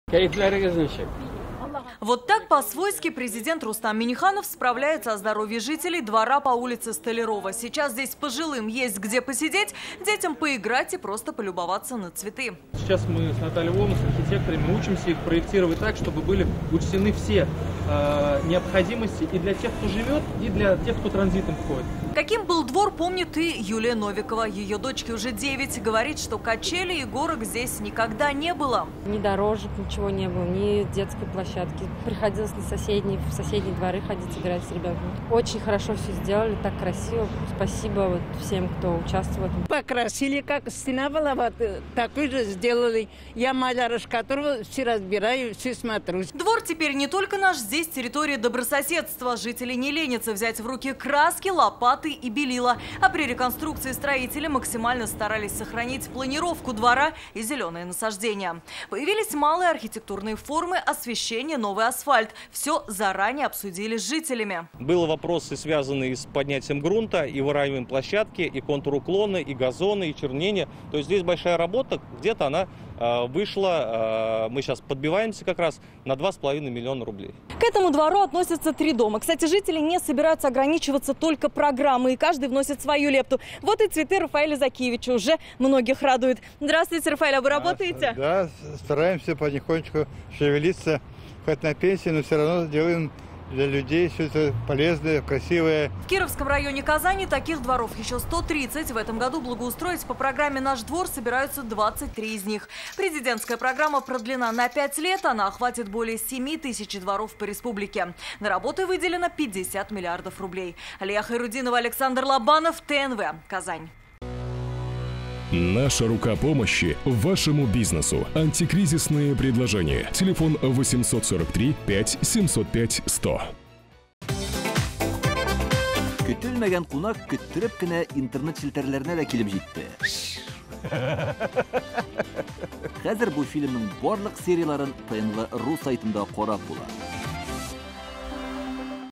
Президентская программа НашДвор меняет отношение людей друг к другу и окружающей среде | ТНВ
Президентская программа "Наш двор" меняет не только облик территорий, но и отношение людей друг к другу и окружающей среде. Сегодня Рустаму Минниханову показали такой двор, где соседи, пока обсуждали проект, уже вовсю подружились и сейчас даже выходят помогать строителям.

Координаты:
☎ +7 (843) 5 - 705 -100